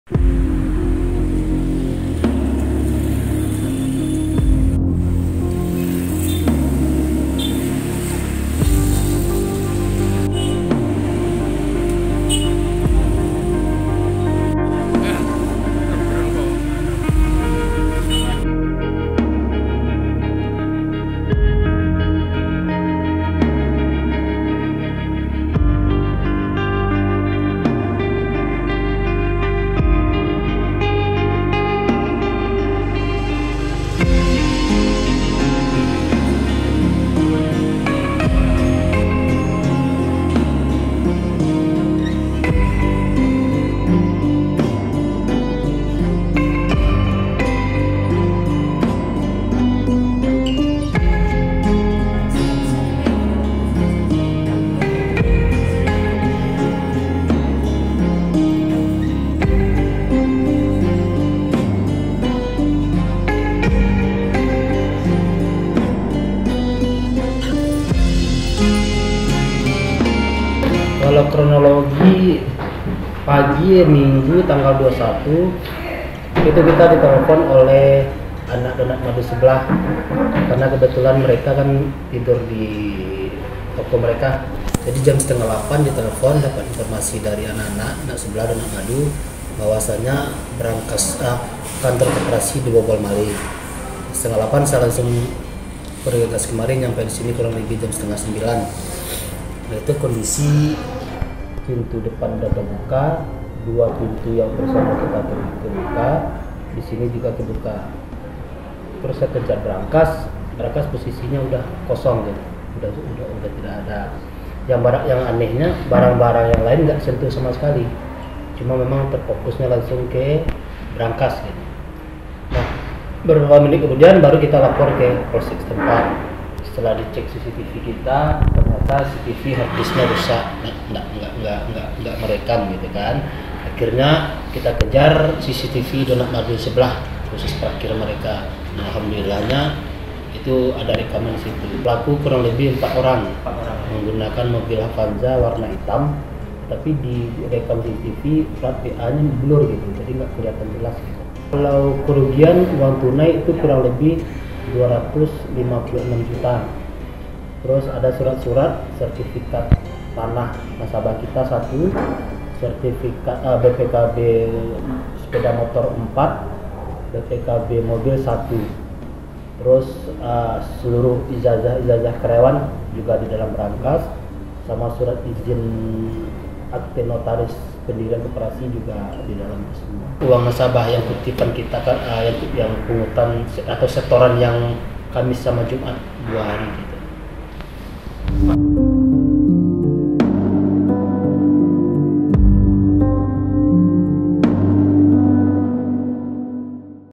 Kronologi Pencurian Brankas Berisi Uang Ratusan Juta Milik Koperasi di Kota Padang
Koperasi Serba Usaha Dewantara Ranah Minang yang berlokasi di Jalan Dr Soetomo, Kecamatan Padang Timur, Kota Padang, Sumatra Barat (Sumbar) dibobol maling. Satu unit brankas berisikan dokumen dan uang ratusan juta raib.